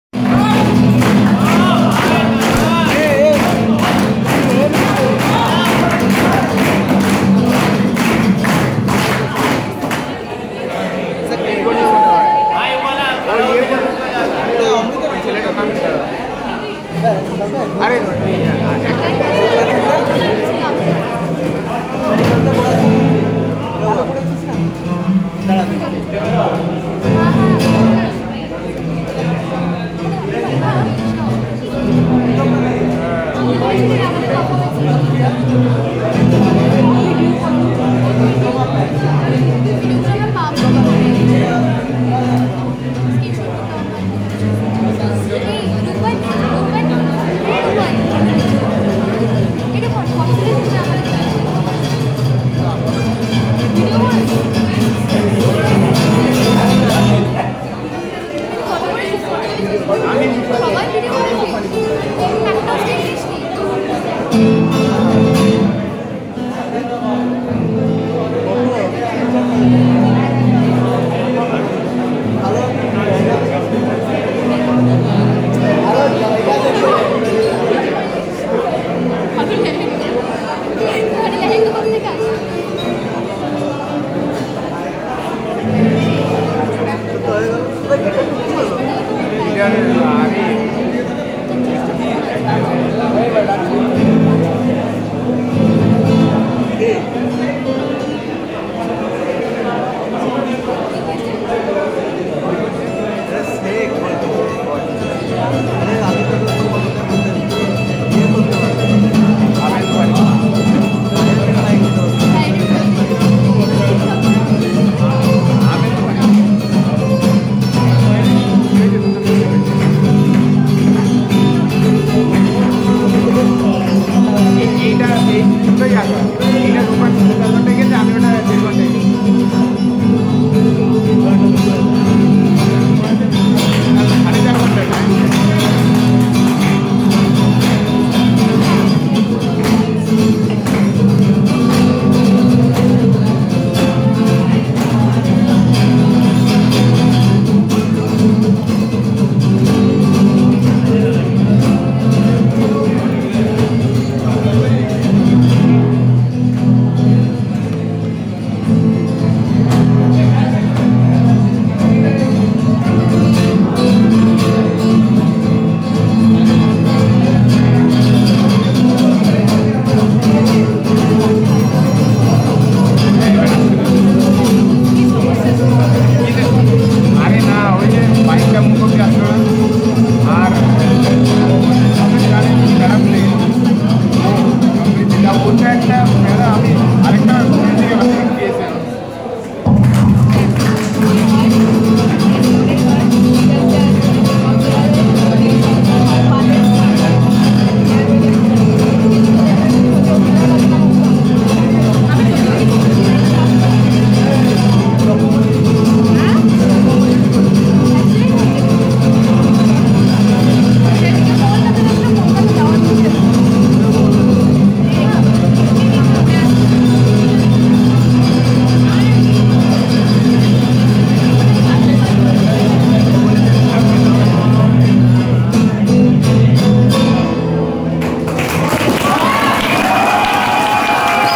Solo Guitar Playing by Abhik Das | Fresher's Day 2023 | Kazi Nazrul University
Date: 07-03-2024, Gallery 1, SMM Campus
Organized by the School of Mines and Metallurgy and Computer Science
Kazi Nazrul University, Asansol
Managed By: @Active_Soukat